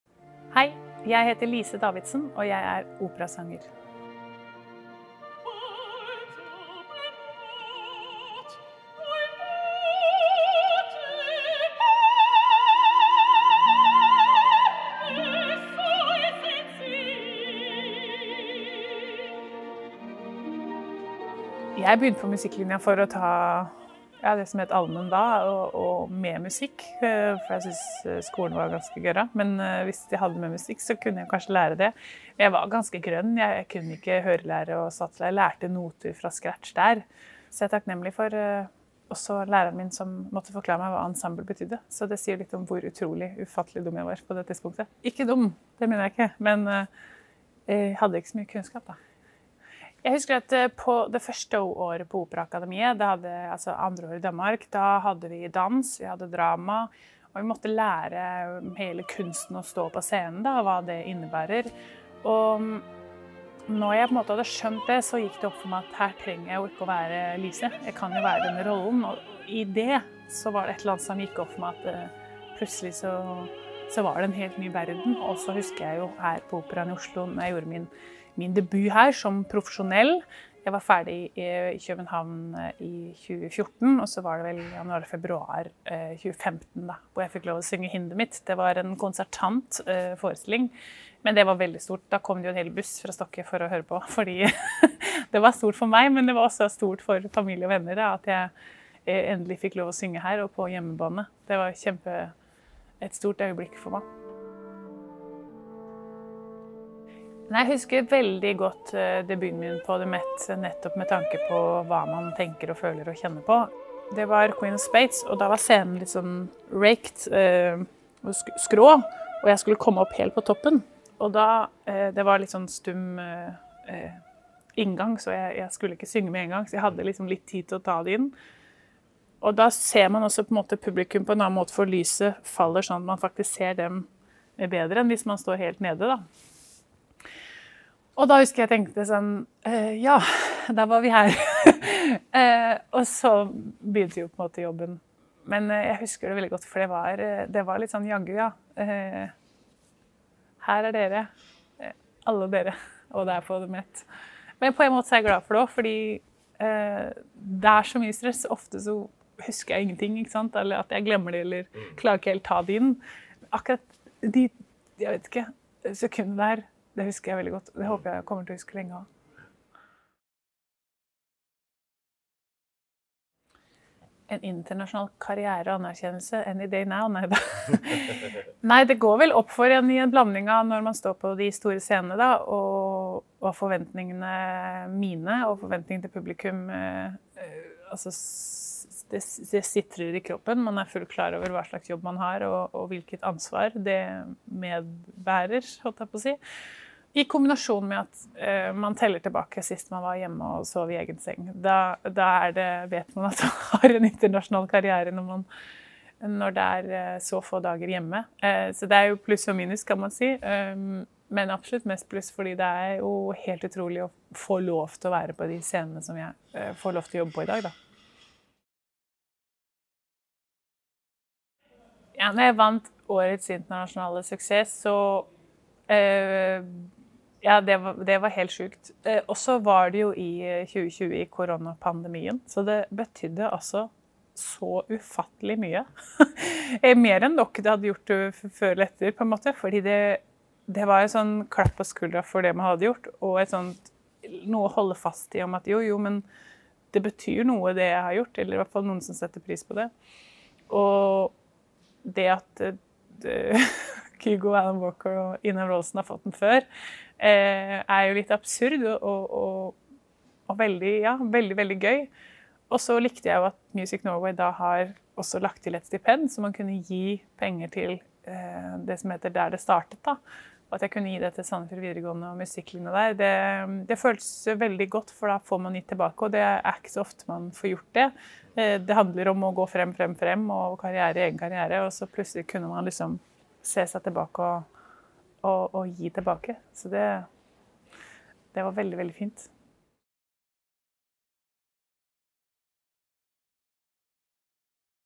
Music Norway 10 år: Lise Davidsen
Music Norway fyller 10 år i 2022, og i den forbindelse har vi tatt en prat med noen av Norges eksportsuksesser. Lise Davidsen er en av Norge og verdens mest suksessfulle stjernesopraner. Her er hennes historie om hvordan veien hennes ble til.

Video er produsert av Sesong 1.
Klipp fra The Metropolitan Opera.

Music Norway is the Norwegian music industry's export facilitator and promotional organization.